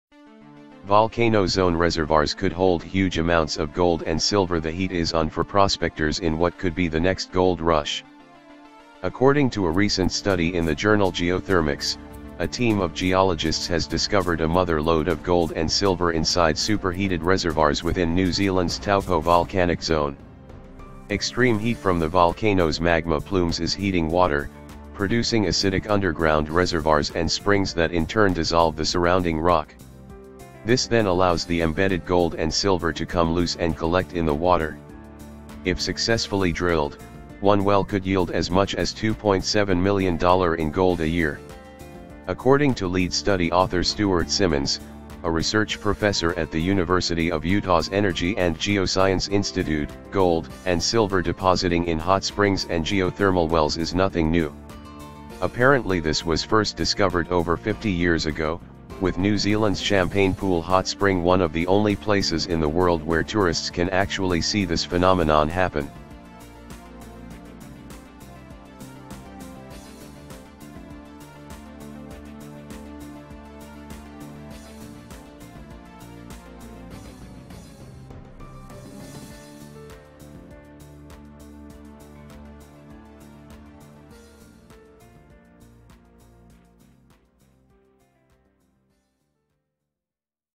Volcano zone reservoirs could hold huge amounts of gold and silver
Volcano zone reservoirs could hold huge amounts of gold and silver – The heat is on for prospectors in what could be the next gold rush. According to a recent study in the journal Geothermics, a team of geologists has discovered a mother lode of gold and silver inside super-heated reservoirs within New Zealand’s Taupo Volcanic Zone. Extreme heat from the volcanoes’ magma plumes is heating water, producing acidic underground reservoirs and springs that in turn dissolve the surrounding rock. This then allows the embedded gold and silver to come loose and collect in the water. If successfully drilled, one well could yield as much as $2.7 million in gold a year.
According to lead study author Stuart Simmons, a research professor at the University of Utah’s Energy & Geoscience Institute, gold and silver depositing in hot springs and geothermal wells is nothing new. Apparently this was first discovered over 50 years ago, with New Zealand’s ‘Champagne Pool’ hot spring one of the only places in the world where tourists can actually see this phenomenon happen.